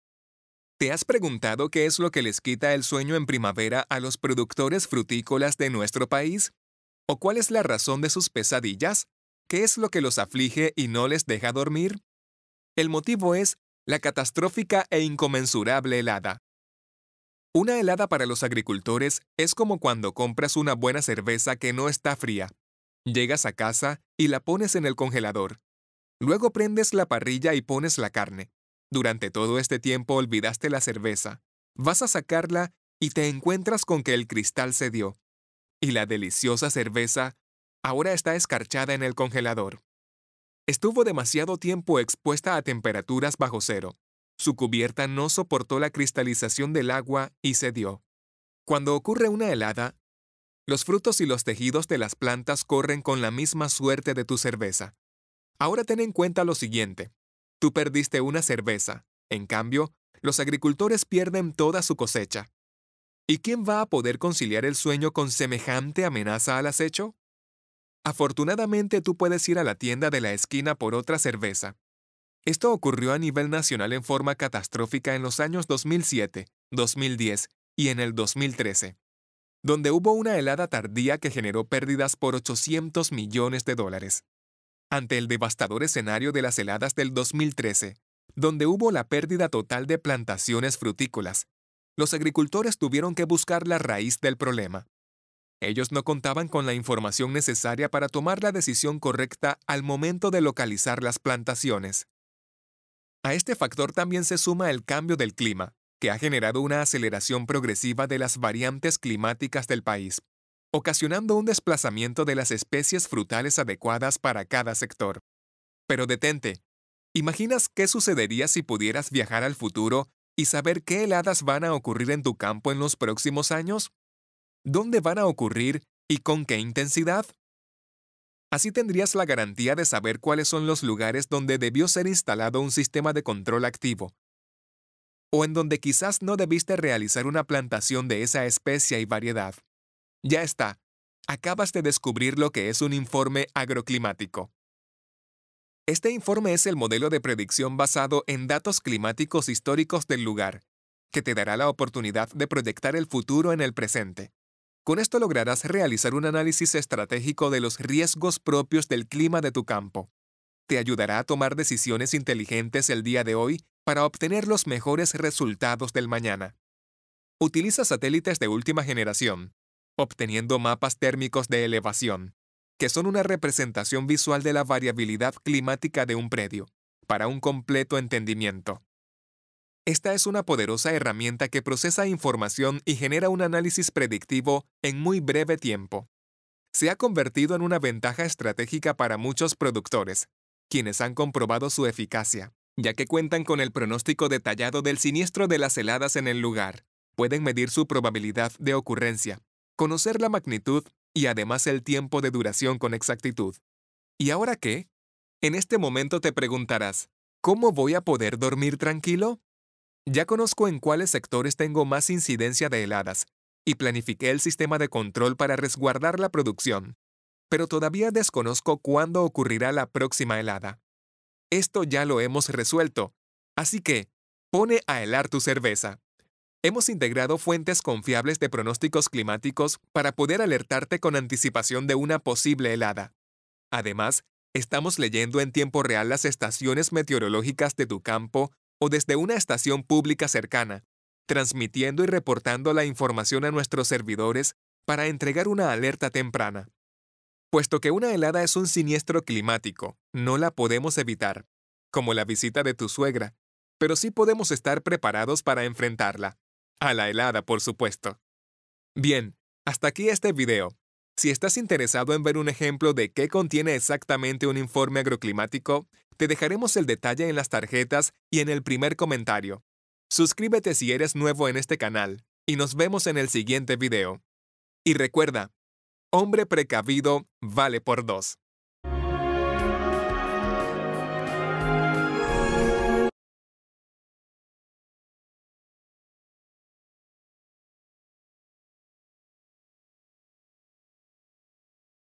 ❄️ ¿Cómo COMBATIR las HELADAS en la agricultura? | ViLab
En este video te contamos qué es un informe agroclimático y cómo te ayudará a definir una estrategia  para enfrentar las heladas de esta temporada. ¡No te lo pierdas!

¿Sabes cuáles son los requerimientos climáticos de los frutales?